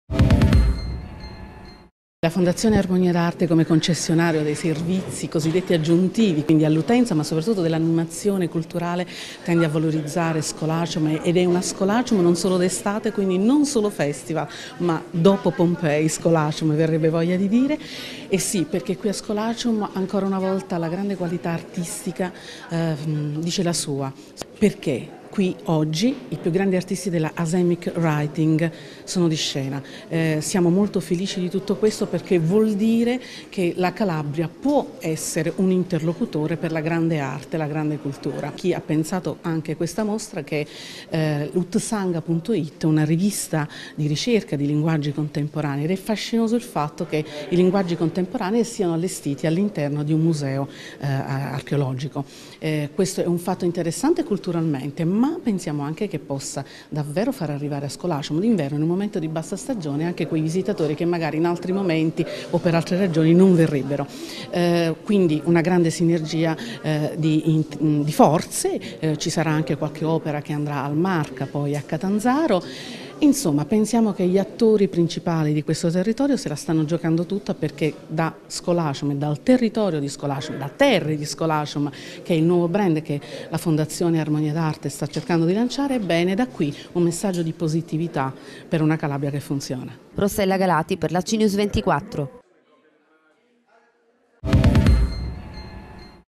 Asemic writing exhibition: “Dopo Pompei c’è Scolacium”
L’arte contemporanea protagonista della mostra “Asemic Writing Exhibition” al Parco Archeologico Scolacium di Roccelletta di Borgia. Una iniziativa della fondazione Armonie d’Arte Festival presieduta da Chiara Giordano.